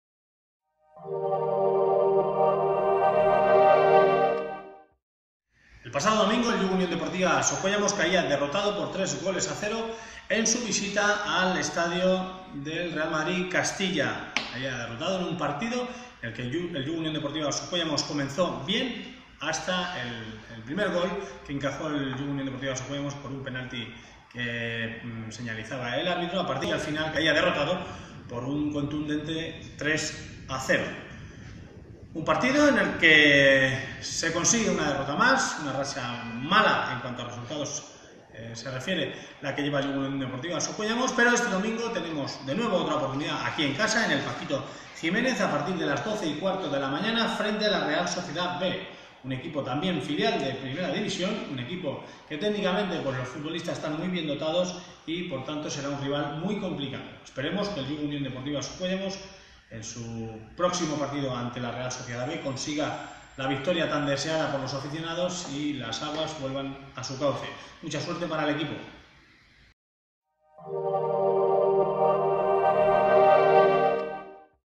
LA OPINIÓN 19 10 2016 MITIN
Una semana más, Mitin analiza la derrota del Socuéllamos ante el Real Madrid Castilla en el Alfredo Di Estéfano y el próximo partido contra la Real Sociedad B el domingo a partir de las 12,15 en el Paquito Jiménez.